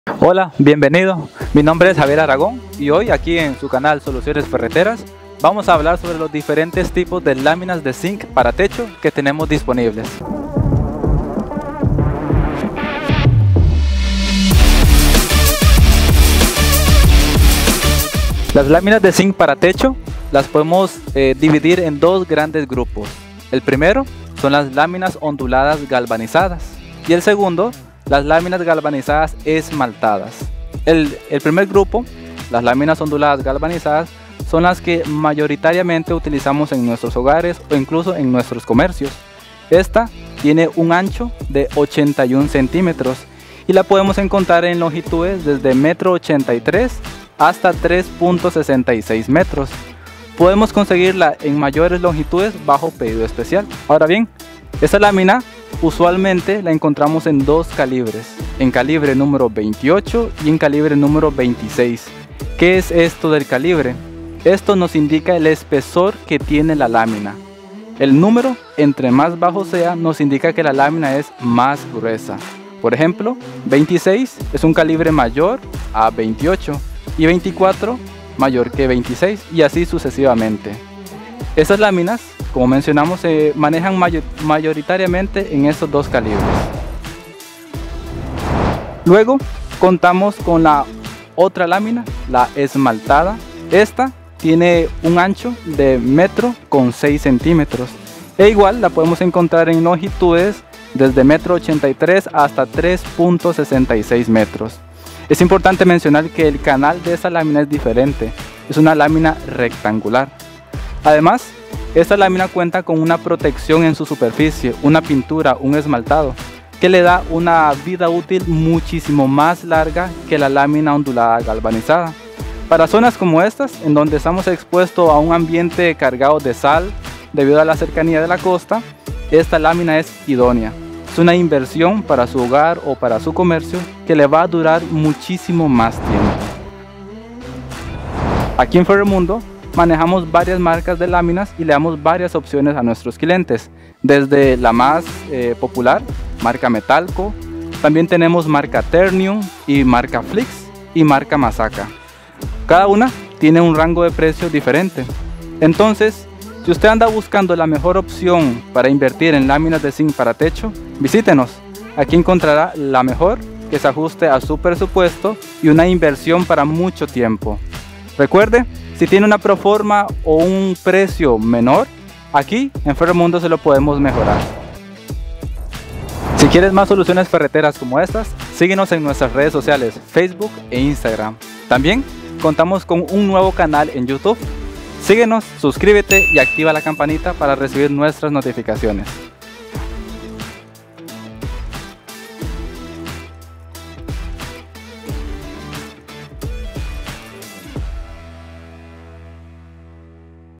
¿QUÉ NECESITO? | TIPOS DE LÁMINAS DE ZINC | SOLUCIONES FERRETERAS
¿No sabes qué tipo de lámina utilizar o cómo escoger correctamente?
Para eso, hoy en Soluciones Ferreteras nuestro amigo Javier Aragón nos brinda esta valiosa información.